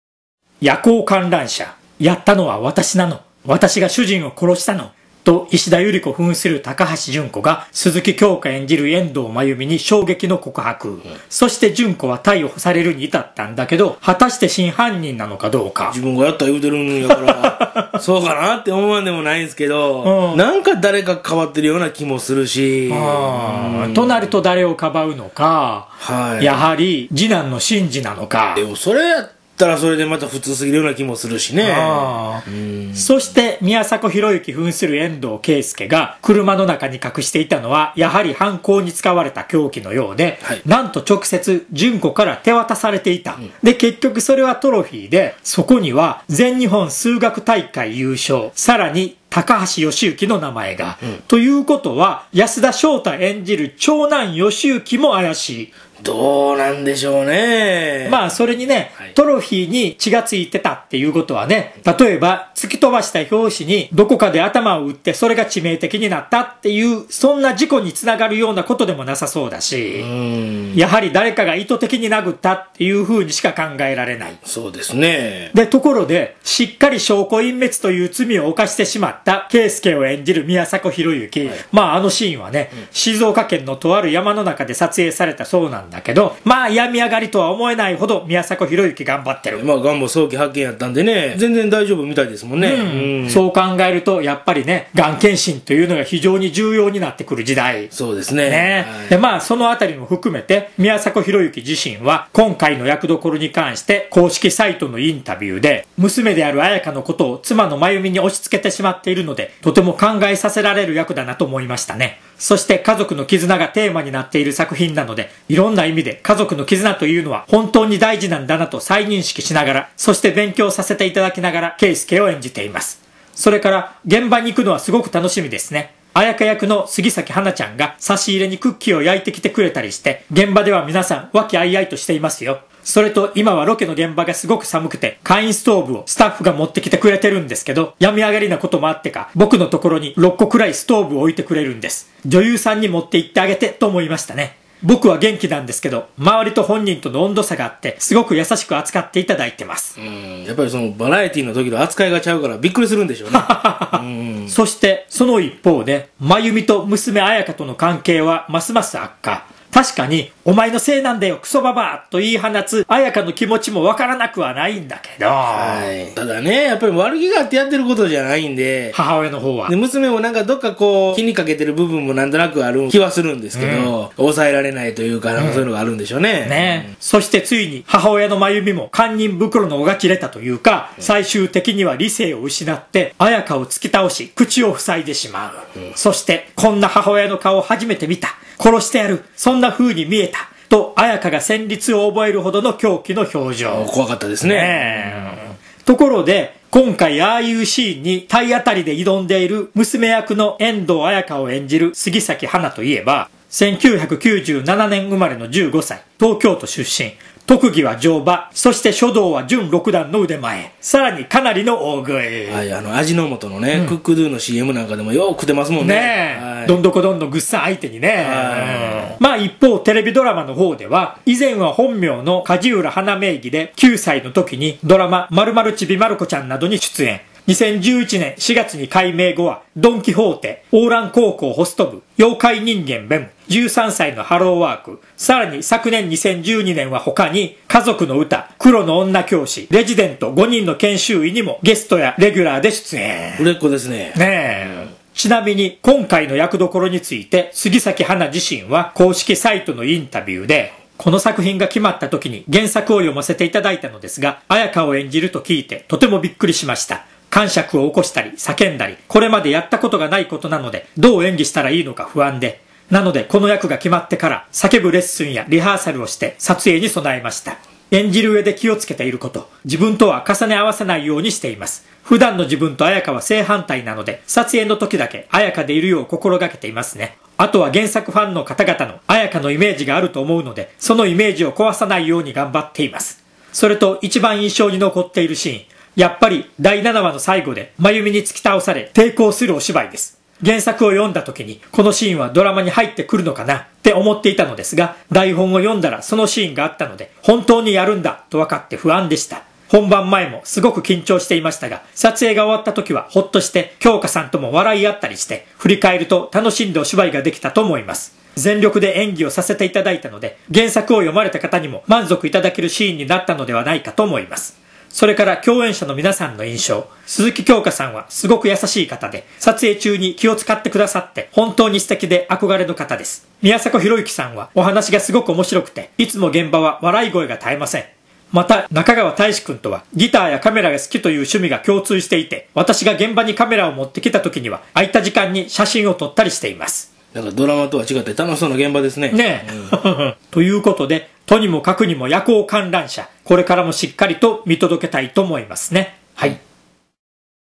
夜行観覧車：放談！その５ ＠ 「テレビ番組を斬る！」
「夜行観覧車」宮迫博之＆鈴木京香＆杉咲花「家族」
「３月７日」のテーマ！ドラマ「夜行観覧車」その５